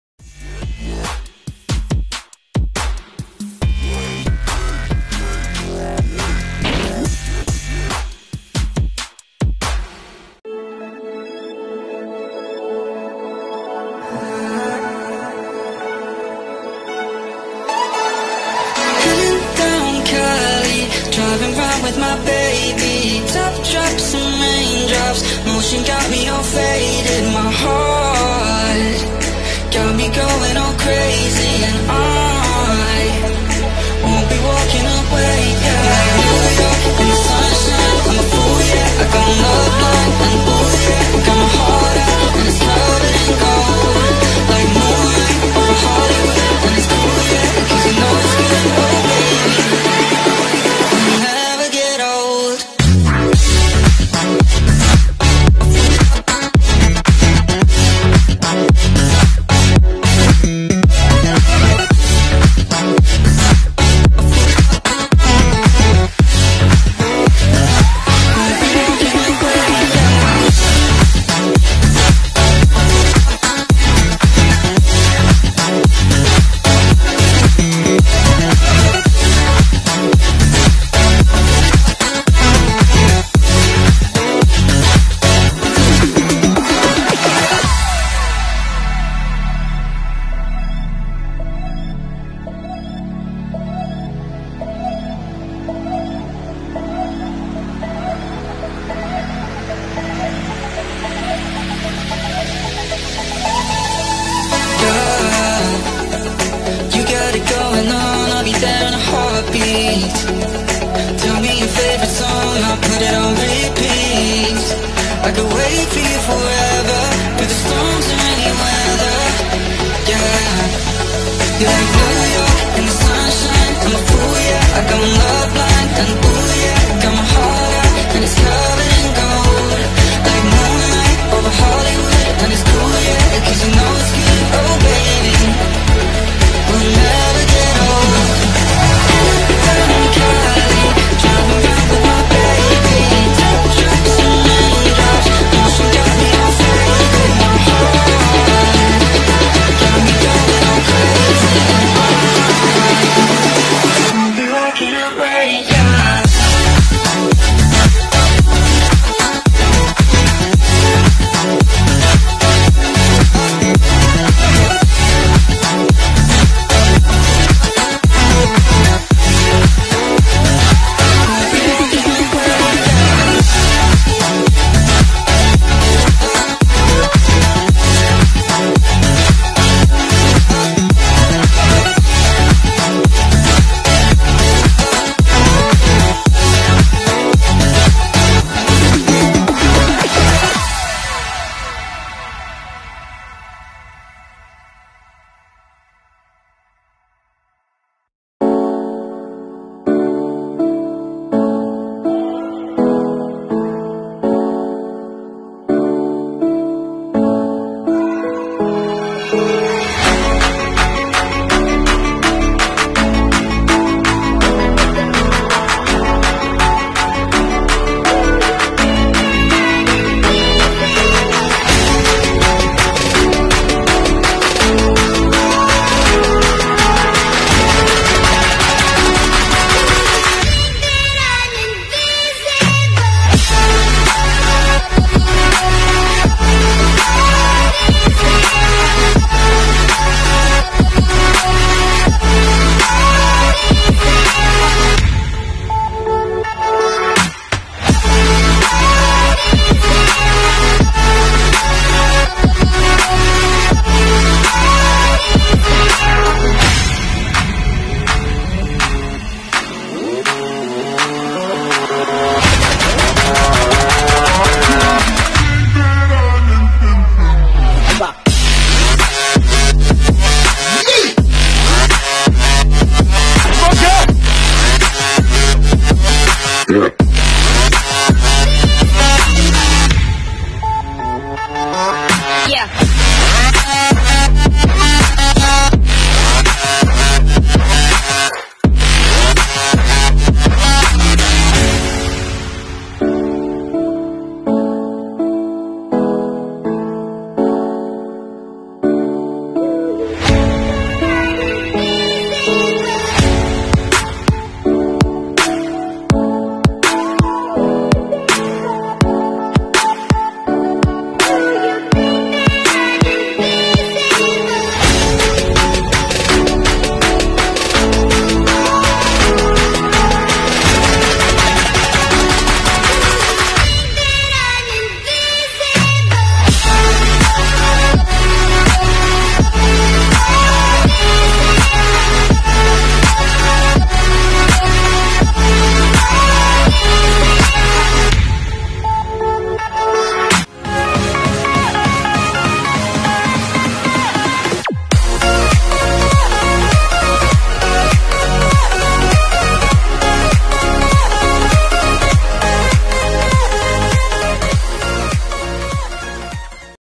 Samsung Galaxy A71 5G & A51 5G 2020 || Official First look & Specifications || Exynos 980 5G, Price
© KV9TECH